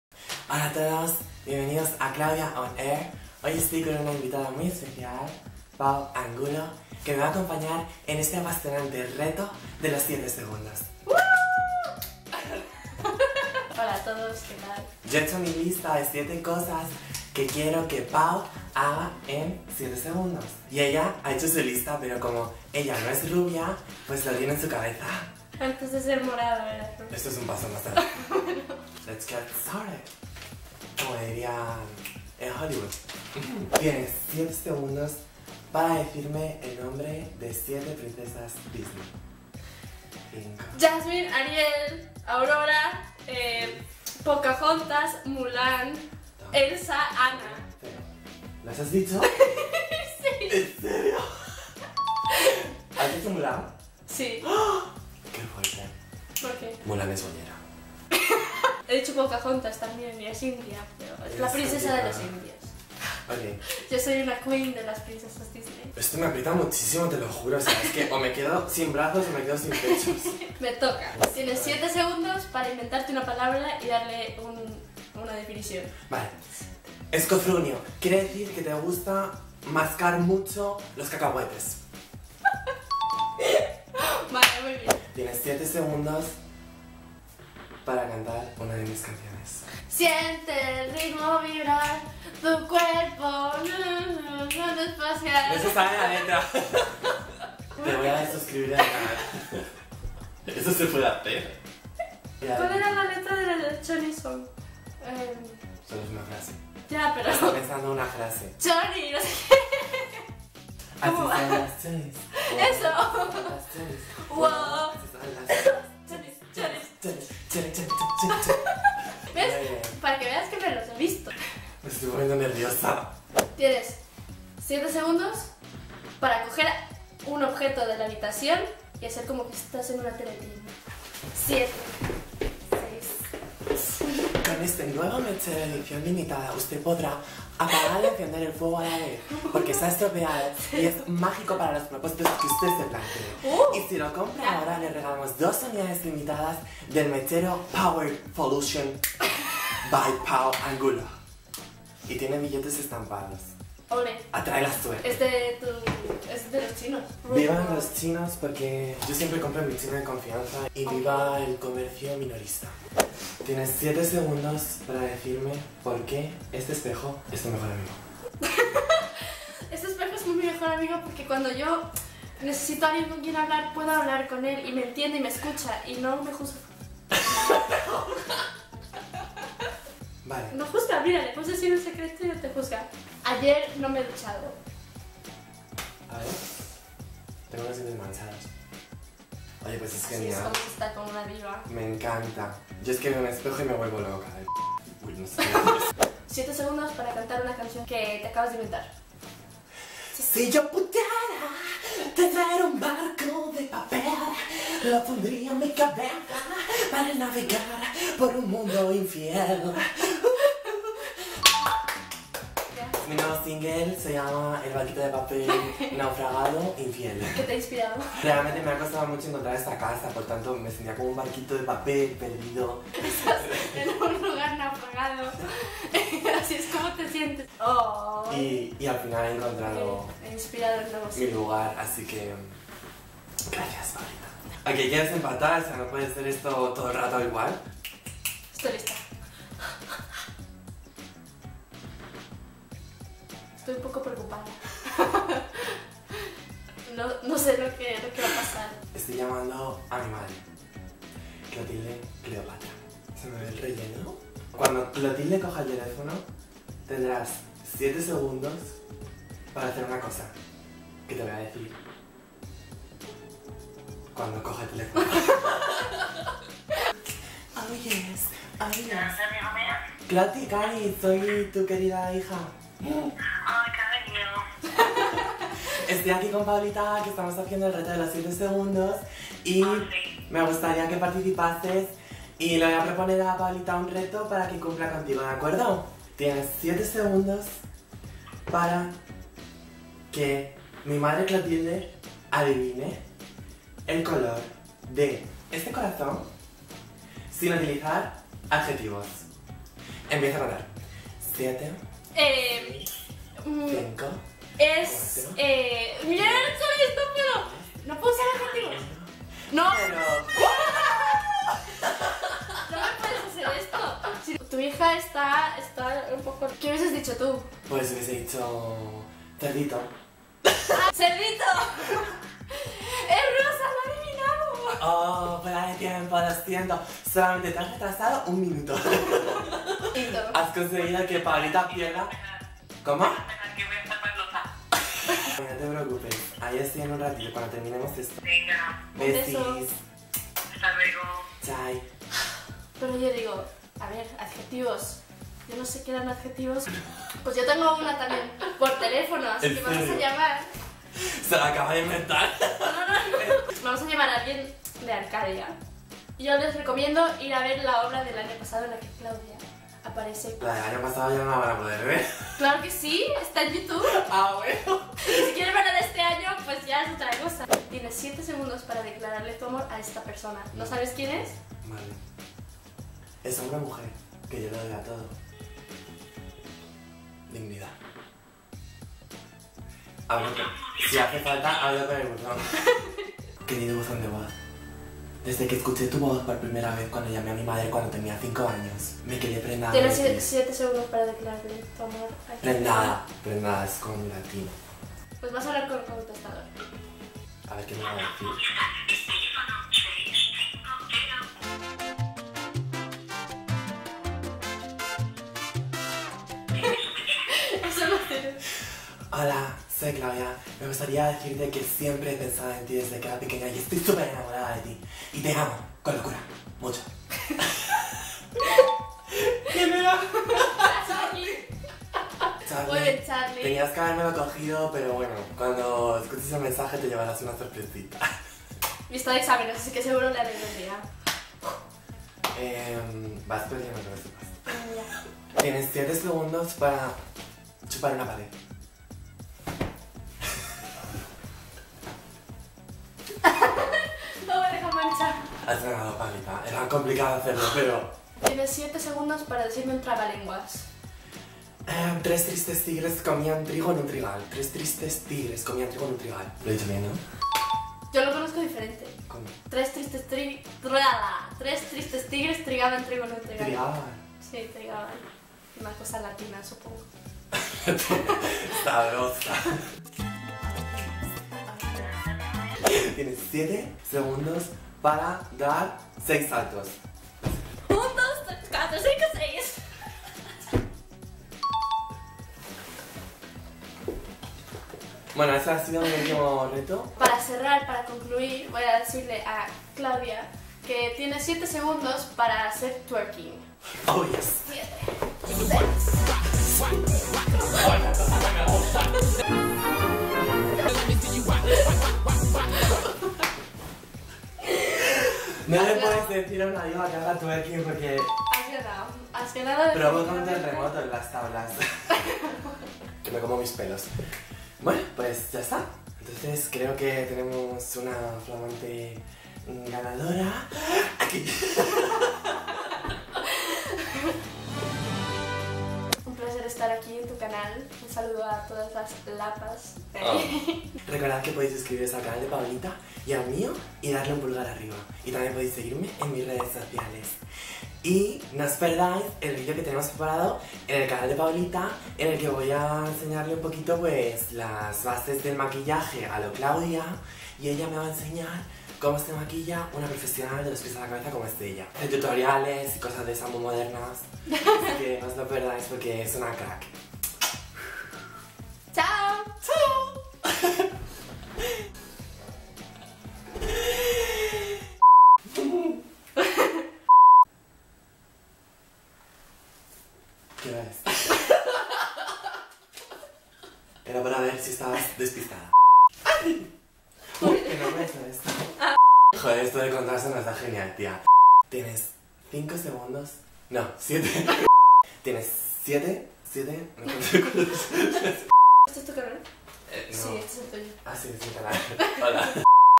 RETO DE LOS 7 SEGUNDOS con Pao Angulo
Claudia Afrodita tiene el honor de acoger a Pao Angulo por un día en su canal para hacer el RETO DE LOS 7 SEGUNDOS. Podrás ver a Pao Angulo nombrar princesas Disney en tiempo récord, a Claudia inventar un hit por el camino y hasta a Clotilde Cleopatra vía telefónica colaborando en los retos... ¡Ah! Y una sesión express de TWERK incluida. Tampoco te pierdas a Claudia y Pao maquillándose mutuamente en el vídeo de su canal: